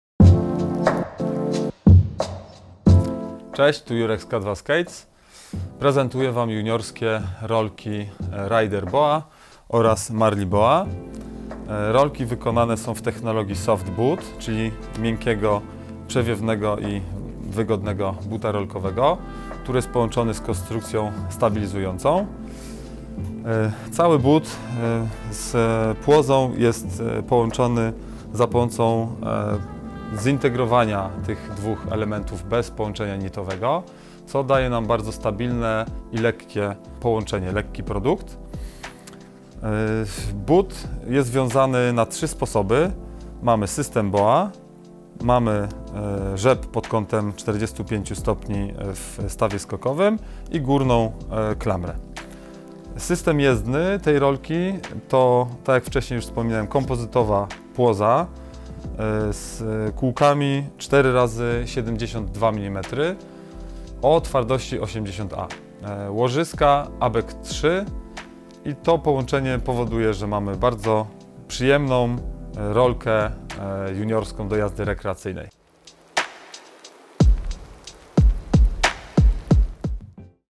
Rolki dziecięce K2 MARLEE RAIDER BOA
Rolki K2 Marlee / Raider BOA to dziecięce rolki z regulacją przeznaczone do jazdy rekreacyjnej. Regulowany rozmiar w zakresie 5 numerów umożliwia dopasowanie rolek do rosnącej stopy dziecka. Wygodny, oddychający but. Rozmiar kół oraz ich parametry dostosowane dla dzieci. Kompozytowe płozy bardzo dobrze amortyzują nierówności. Błyskawiczny system wiązania i dopasowania BOA ułatwia zakładanie rolek. Rolki posłużą dziecku przez kilka sezonów.